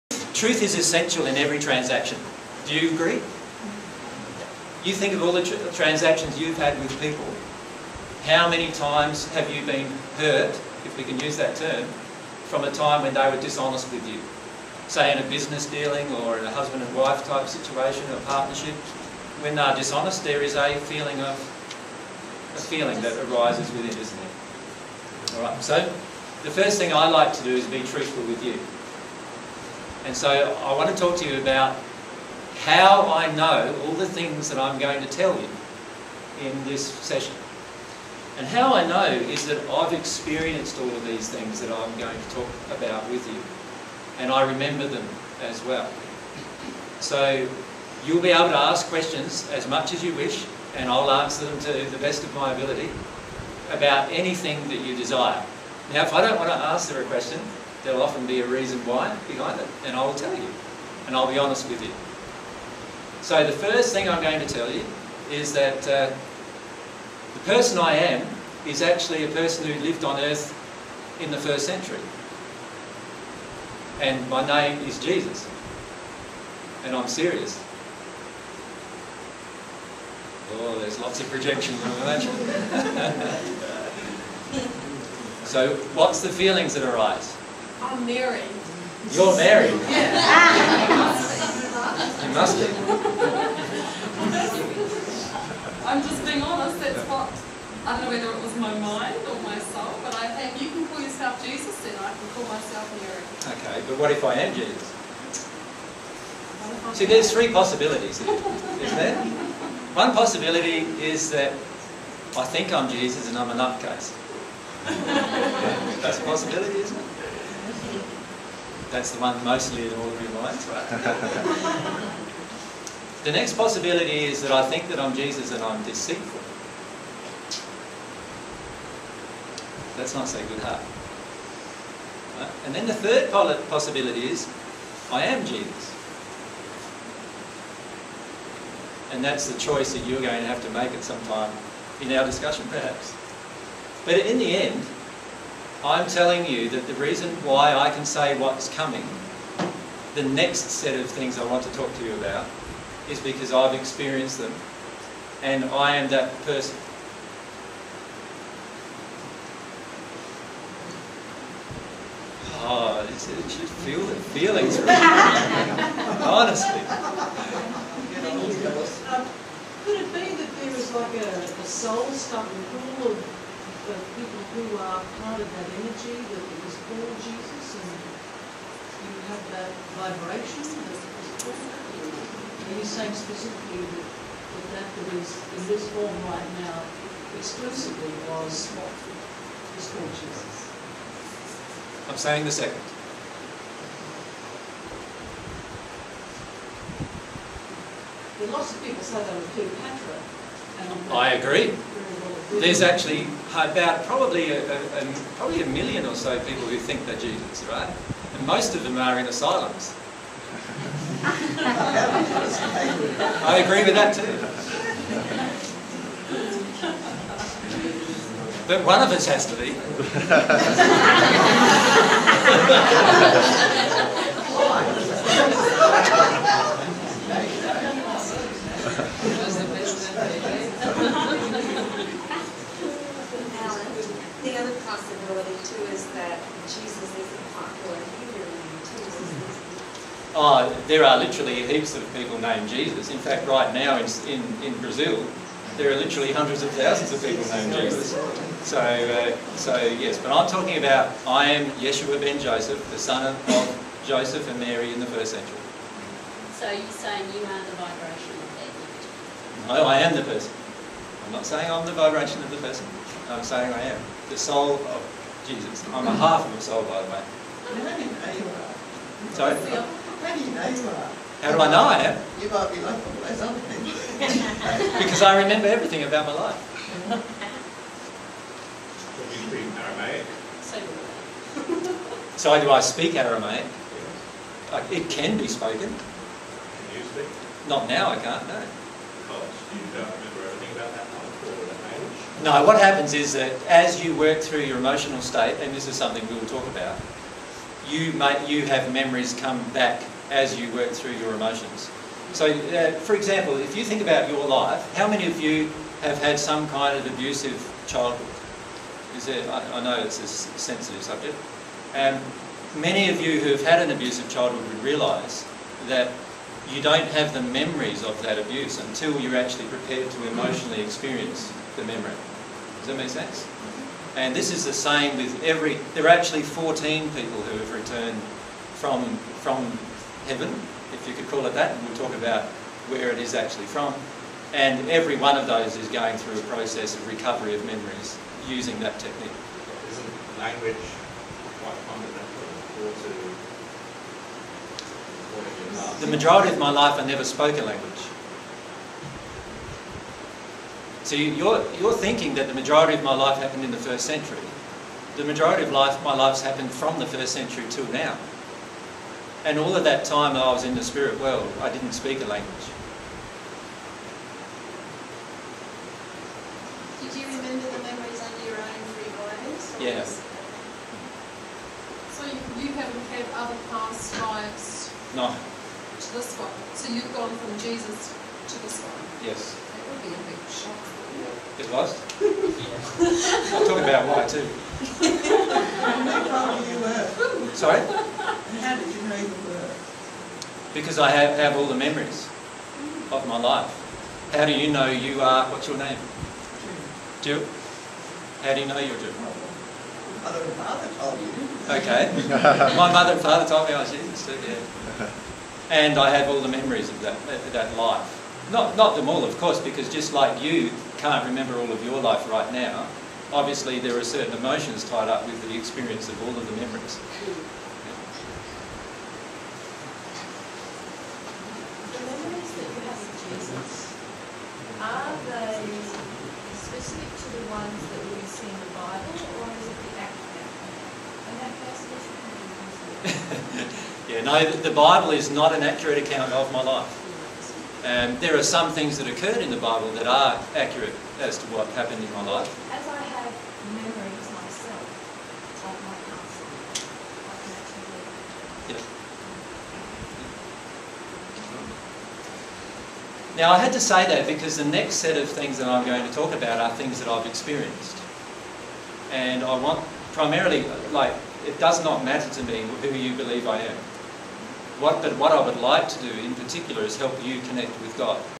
Jesus on Thinking, Believing and Knowing He Is Jesus
Jesus discusses owning his identity and remembering who he is.

This is a section of the presentation by Jesus entitled "20080120 Overview Of Divine Truth - Secrets Of The Universe".

dtstart:001834:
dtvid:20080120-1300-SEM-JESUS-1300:
dtmd5:2c39035f3a74228556fd50f089a90853: